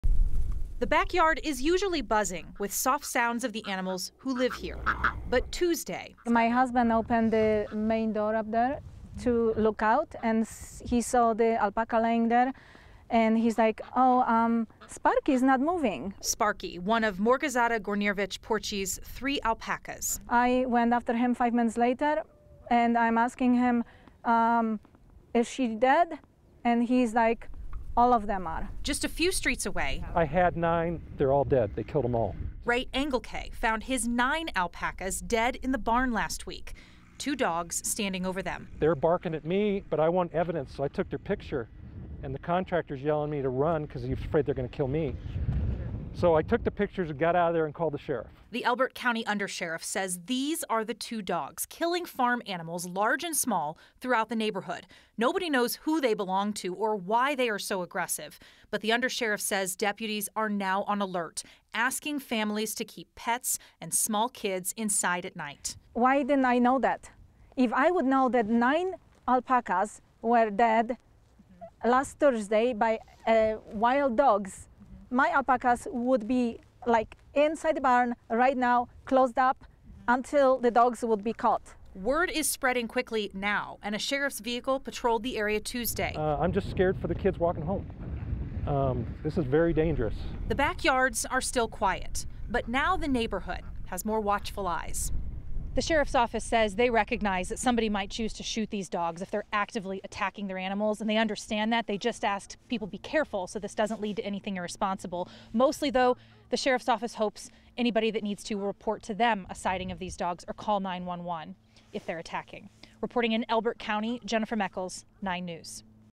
Colorado residents on-edge after 12 alpacas, 4 goats killed by dogs
The Elbert County Sheriff’s Office is looking for two dogs that they think have killed more than a dozen farm animals in the past two weeks, including at least 12 alpacas and four goats.

9NEWS (KUSA) is located in Denver, Colorado.